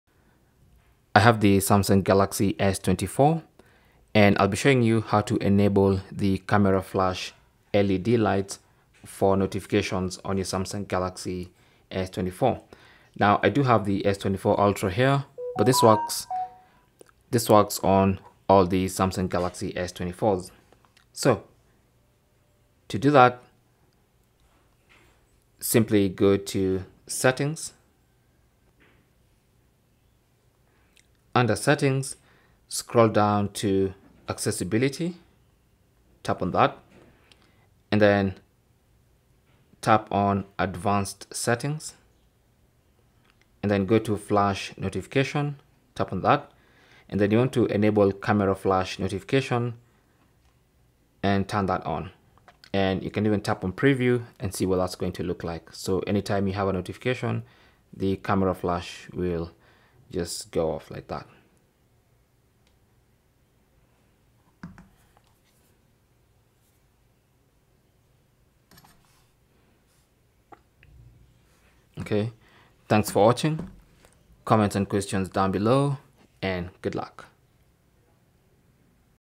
Samsung Galaxy S24/ S24 Ultra - How To Enable Camera Flash Notification Light
Learn How To Enable Camera Flash Notification Light On Samsung Galaxy S24, S24+ or S24 Ultra.

Welcome to our step-by-step guide on activating the camera flash notification light feature on your Samsung Galaxy S24, S24+, or S24 Ultra!

In this tutorial, we'll walk you through the simple process of enabling the camera flash to notify you of incoming calls, messages, and notifications. This handy feature not only adds a touch of personalization to your device but also ensures that you won't miss any important alerts.

How To Enable Camera Flash Notification Light On Samsung Galaxy S24, S24+ or S24 Ultra

1. Go to Settings app. Scroll and select Accessibility option. Tap on Advanced settings.

2. Navigate to Flash Notification. Enable Camera Flash Notification by tapping on the toggle next next to it. Tap on the preview button to see what it looks like.

Samsung Galaxy A15 (Not final link)

Cell Phone Tripod Adapter